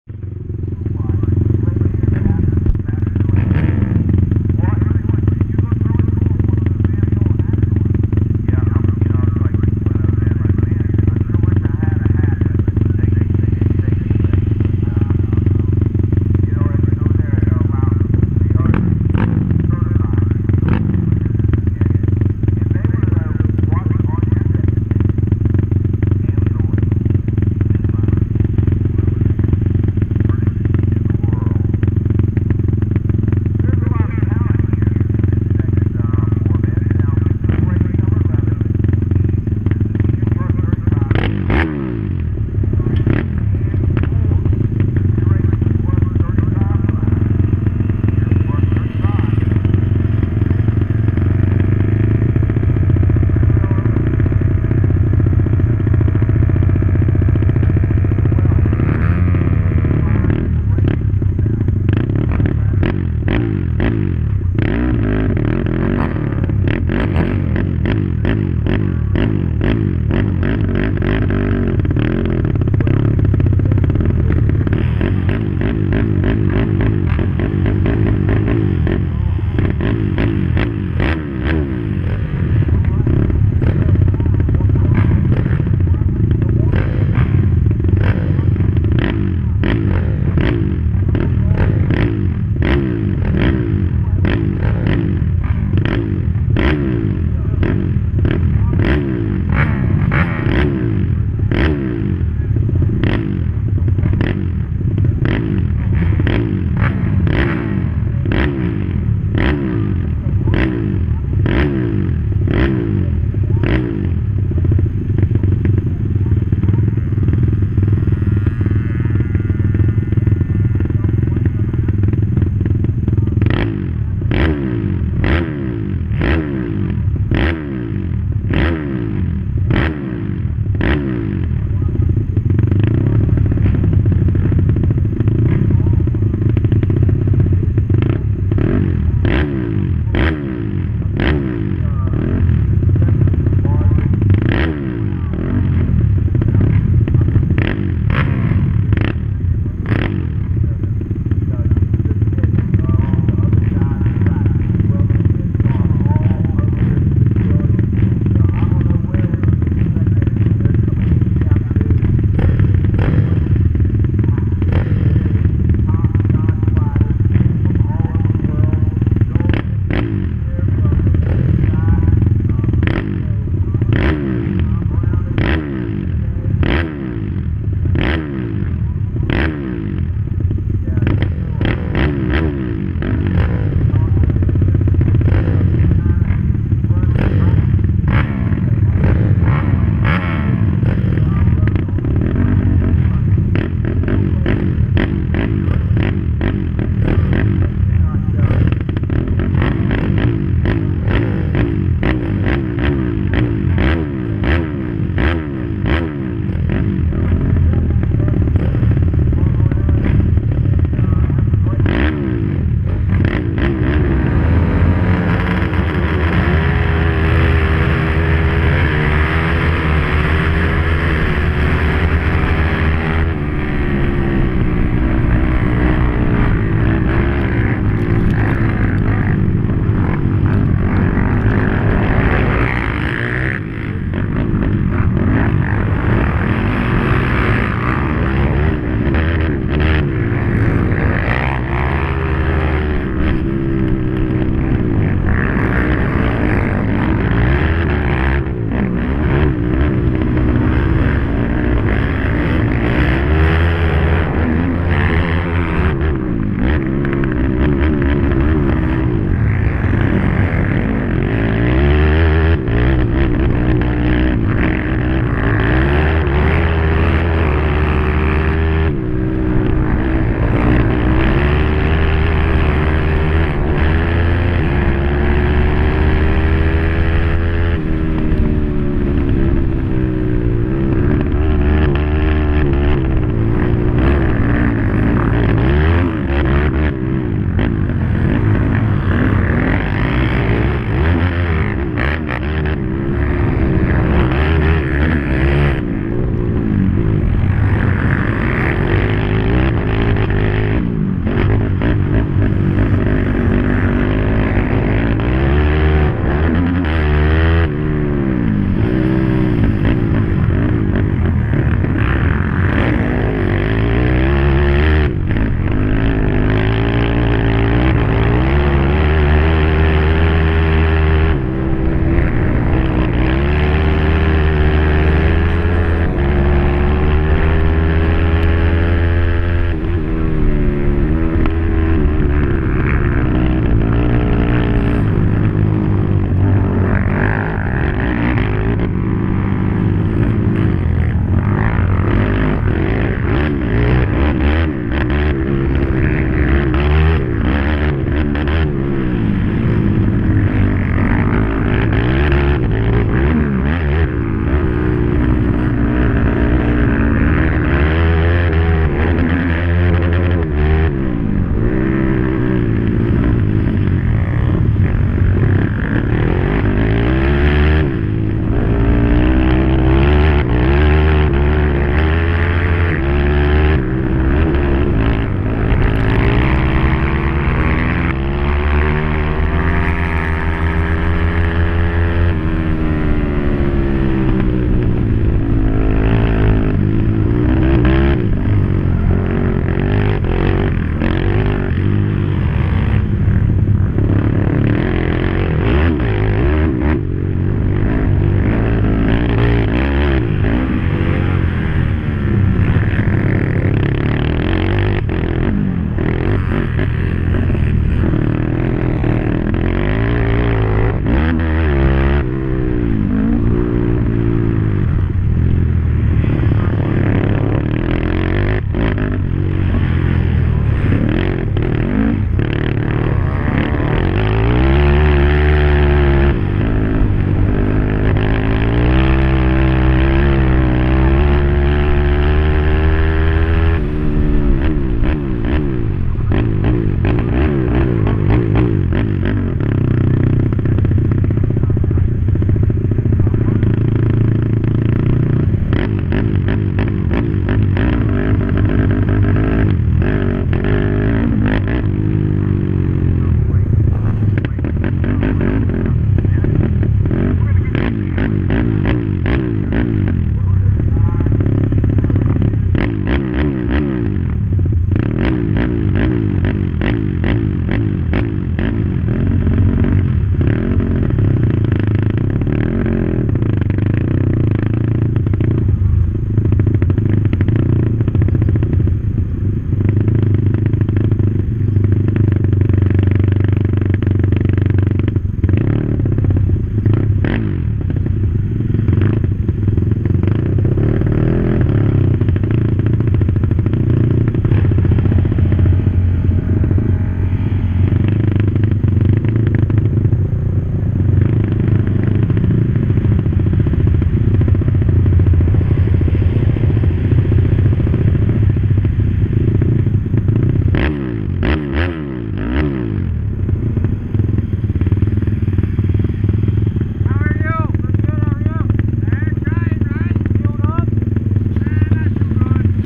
7-27-19 NEEDT Senior 35+ Heat 1 (Shawn) Round 5 Pine Lake Raceway Ashtabula, Ohio
Senior 35+ Heat 1
1. #807 Bucky Gardner
2. #21 Josh Pangallo
3. #25 Jeremy Holmes
4. #10 Shawn Sadler
5. #77 Jeremiah Sherman
6. #210 Ryan Berkey
7. #416 Alvin Marcum
8. #44 Bryan Robertson
9. #00 Rich Schoberl
10. #156 Sylvain Arseneault
11. #37 Mark Gary
12. #133/ Mark Rich